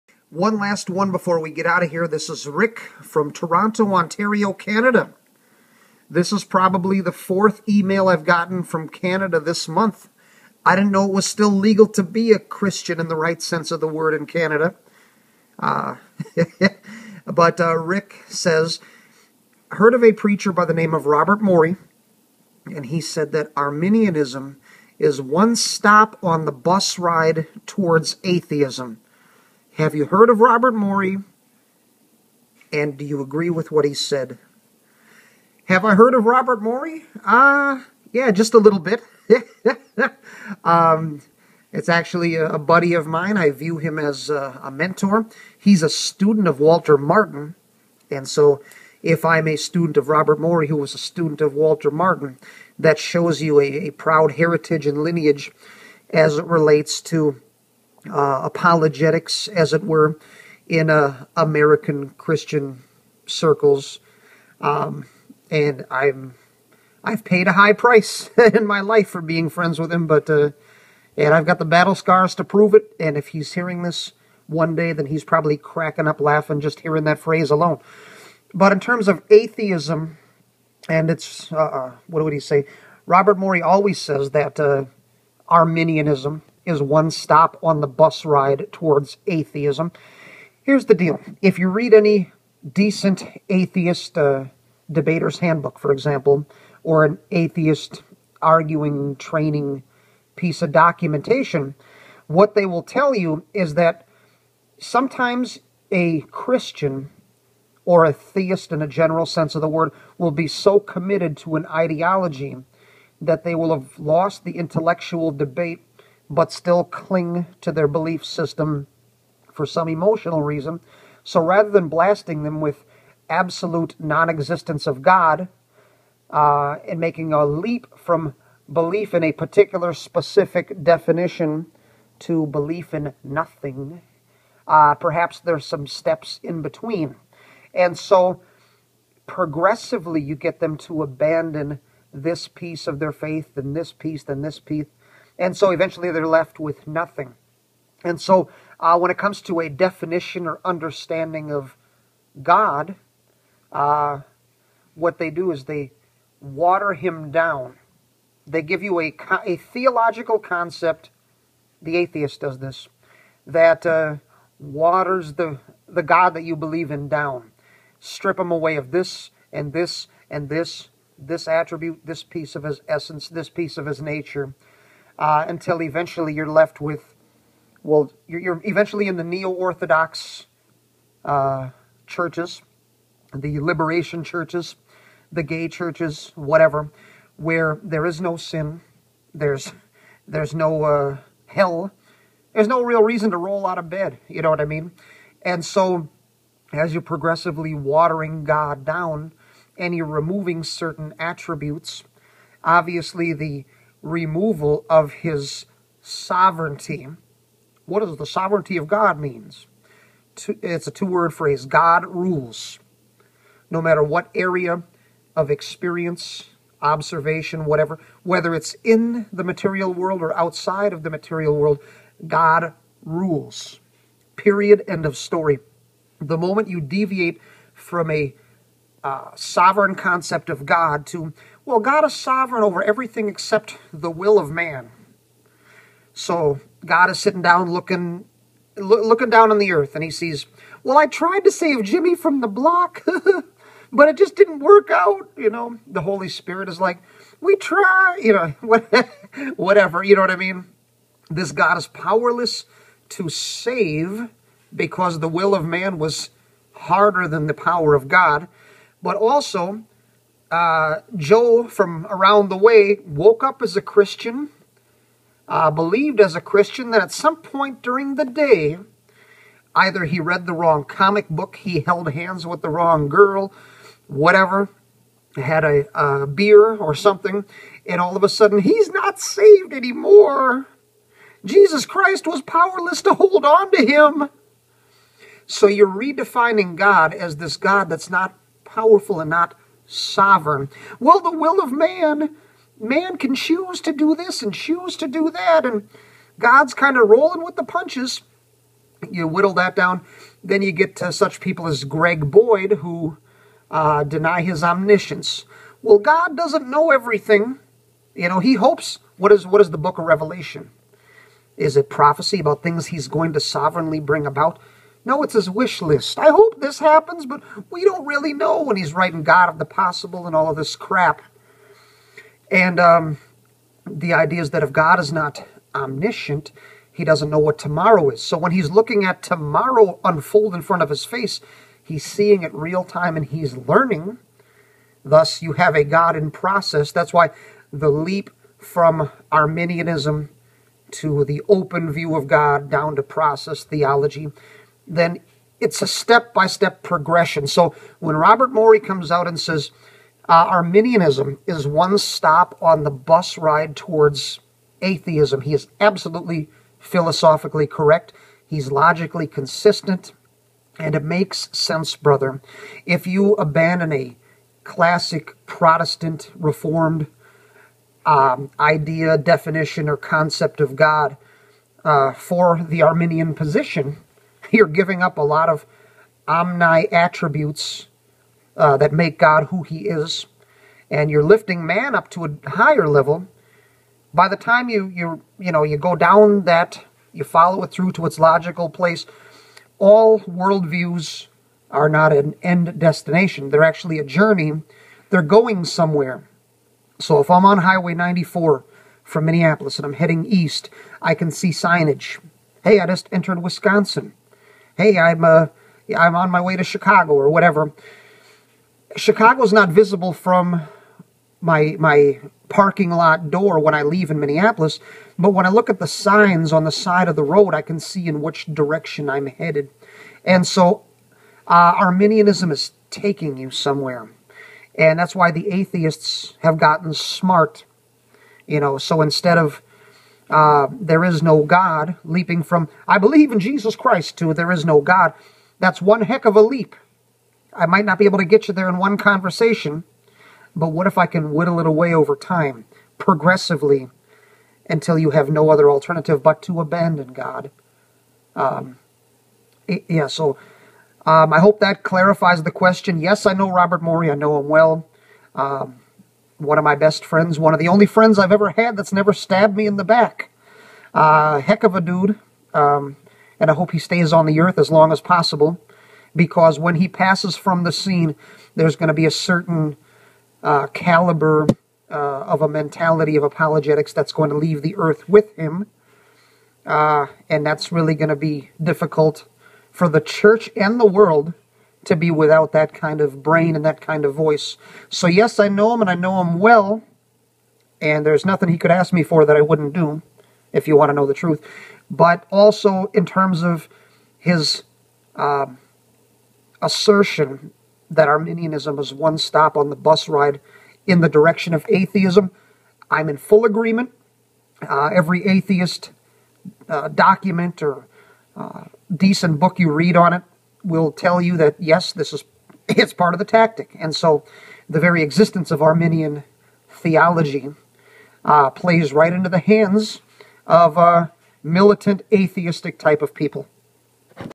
SHANLEY ON ROBERT MOREY'S CLAIM: "ARMINIANISM IS ONE STOP ON THE BUS RIDE TOWARDS ATHEISM"
Dr. Robert Morey, author of "THE NEW ATHEISM AND THE EROSION OF FREEDOM" and Reformed Theologian, has famously went on record as claiming, "Arminianism is one stop on the bus ride towards atheism".  A listener to the work of "SOVEREIGN GRACE INSTITUTE OF APOLOGETICS, THE MINISTRY OF BRIAN JAMES SHANLEY" sent in an email asking our thoughts on that statement of Morey's.  Here is Shanley's response to the listener's email inquiry.